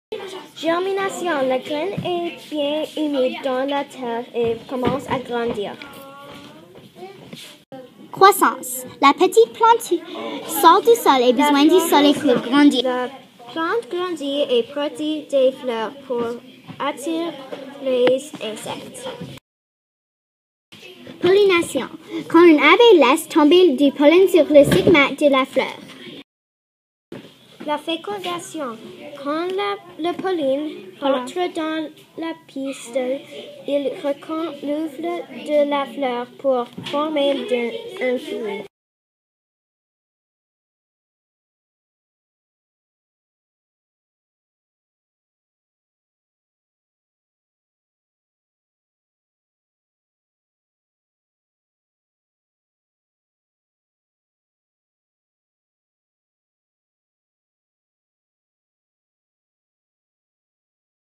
Alora, Mustapha et Emma
La croissance des plantes stop motion par la classe de M Muise.